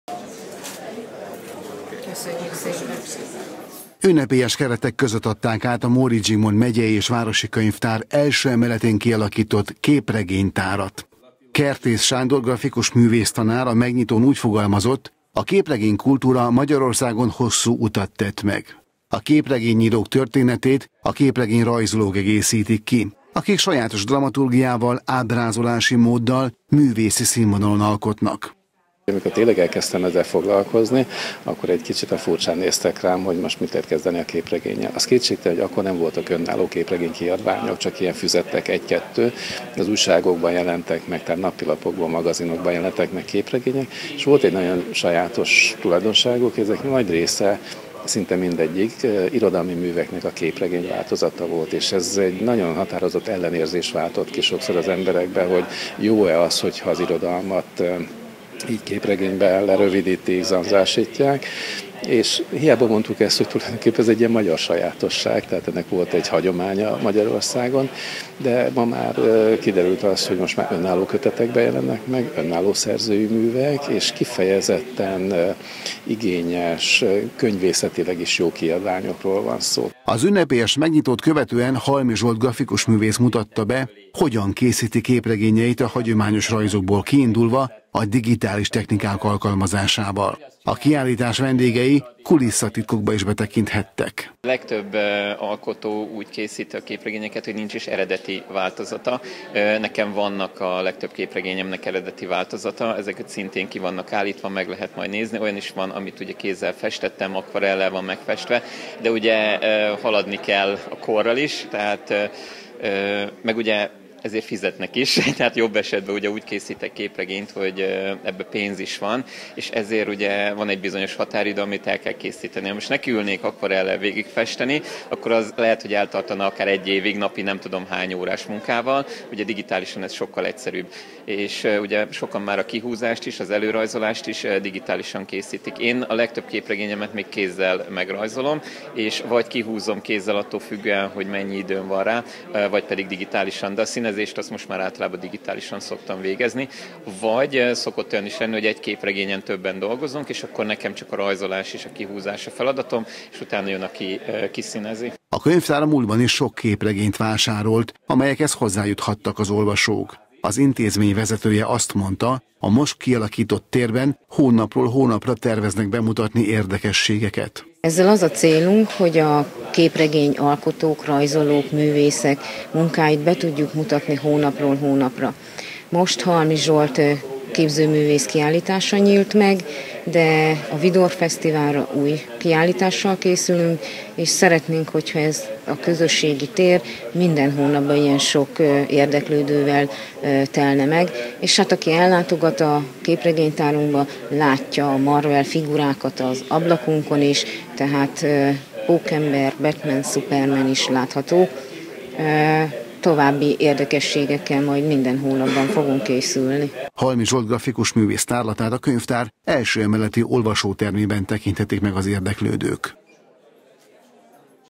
Képregénytárlat
Képregénytárlat nyílt a Móricz Zsigmond Megyei és Városi könyvtárban.
A Képek-Regények-Rétegek című kiállítást Halmi Zsolt grafikusművész több mint húsz alkotásából állították össze, amely a Vidor -fesztivál kezdetéig tekinthető meg. A látogatóknak a megnyitó apropóján Kertész Sándor grafikustanár tartott interaktív tárlatvezetést.